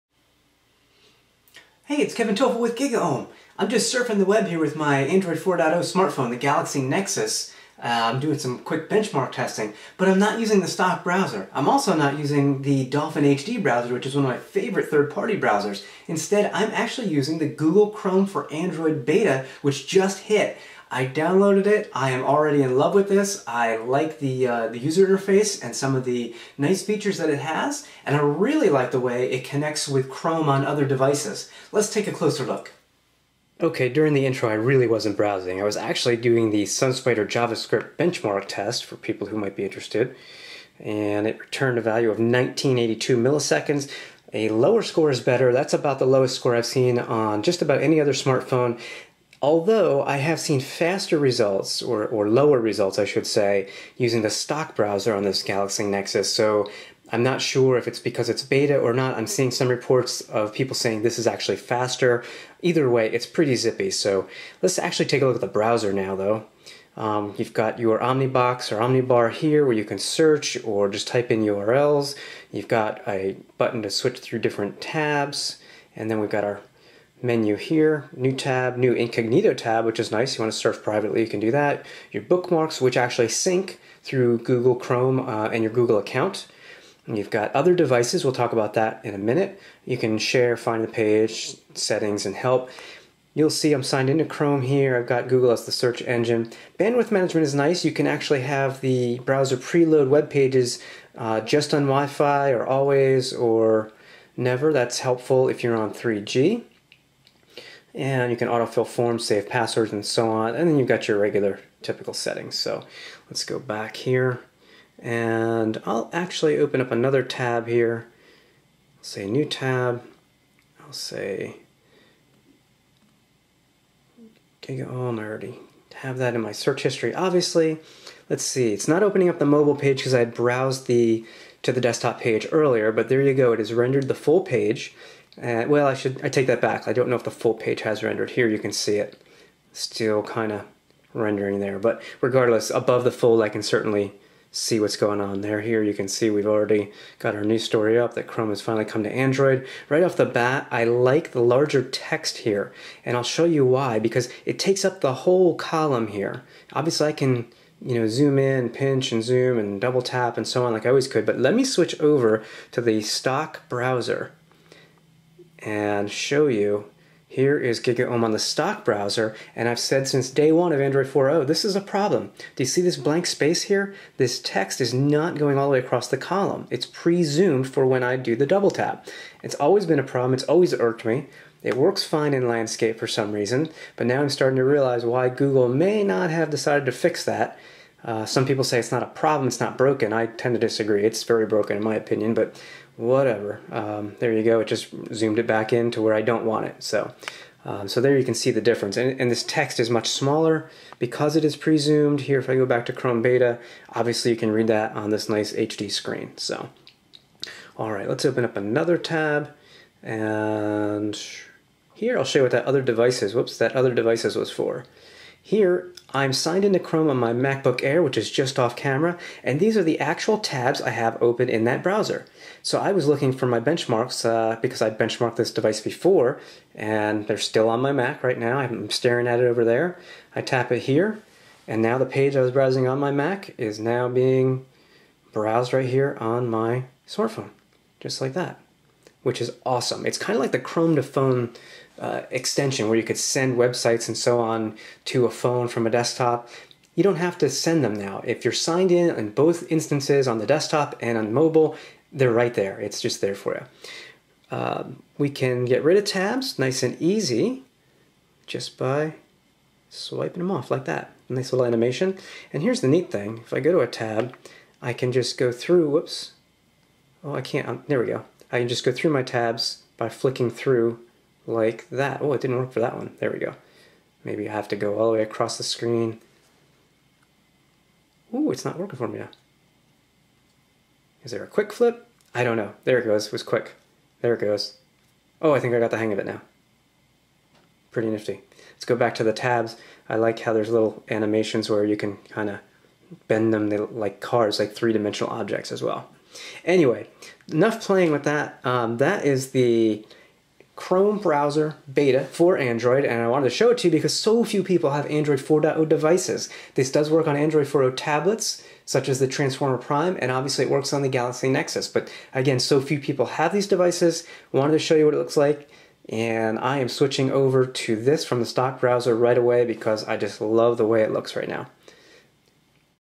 Chrome for Android is faster and feature packed!
Google merged its Chrome browser with Android  today and I'm thrilled to see it. The new Chrome beta is fast and it has a clean interface. If you're a Chrome user on the desktop, Chrome for Android can open whatever webpage you're browsing on the PC.